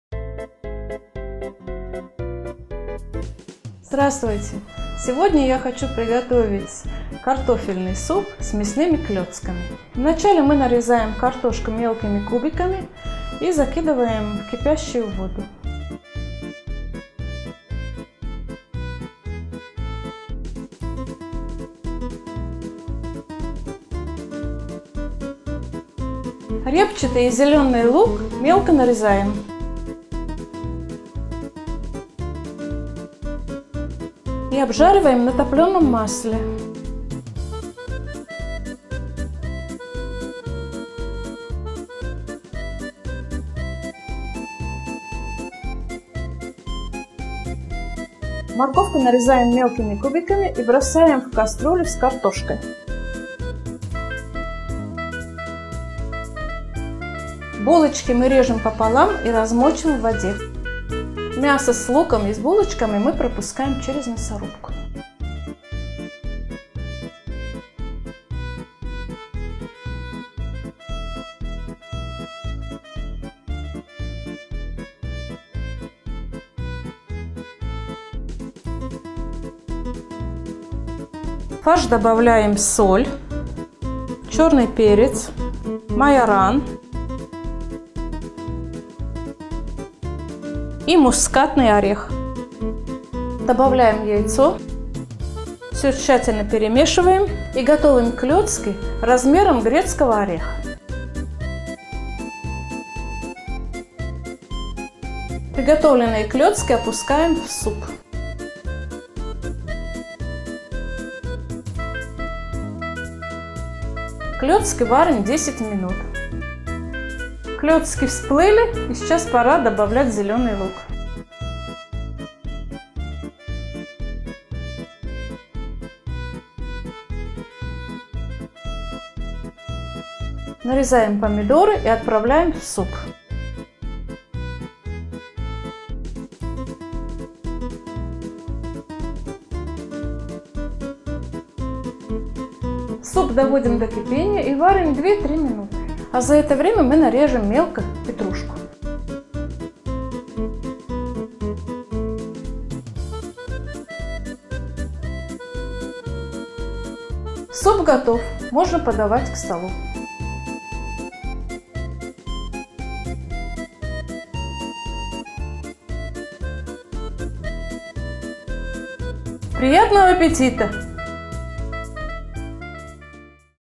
Нежные тефтели и горячий ароматный, наваристый бульон. Как же это все мне нравится!
Нежные тефтели и горячий ароматный наваристый бульон, что может быть лучше на обеденный стол

Картофельный суп с нежными тефтелями
600 г филе индейки
700 г картофеля
2 белых булочки
1 яйцо
4 помидора
1-2 луковицы
2 пучка зеленого лука
1-2 моркови
Мускатный орех, сушеный майоран
Соль, перец
3 ст. л. топленого сливочного масла
Зелень петрушки и укропа

ДОМАШНИЙ РЕСТОРАН® .